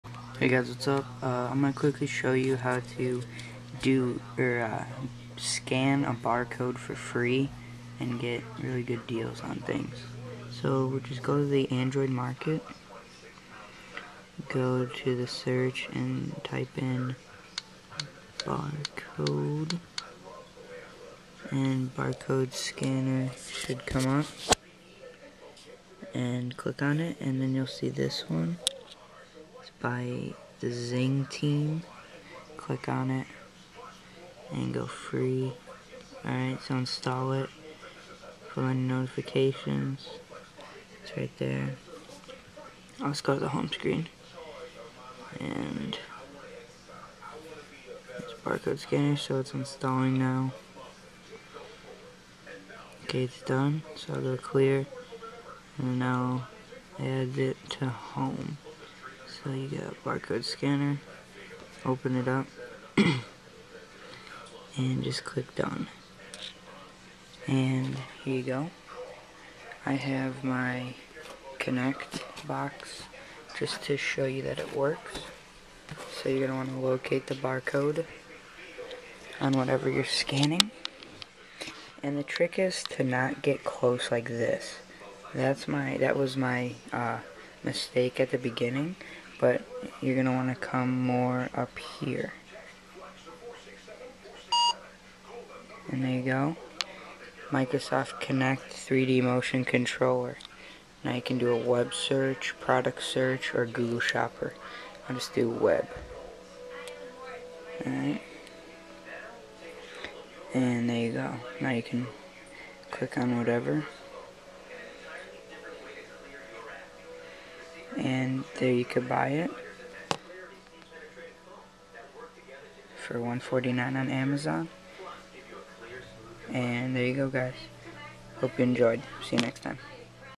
How to Scan Barcodes and Save Money Using an Android Phone
Go to the Android Market on your phone and search 'barcode scanner'. The app used in the video is "Barcode Scanner" and the developer is "ZXing Team".

REMEMBER:::: Don't get to close to the bar code or it won't scan it!!!

Please send me personal messages with video ideas or problems with: Xbox 360, Apple iDevice, Windows computers, Mac computers, or Android based phones.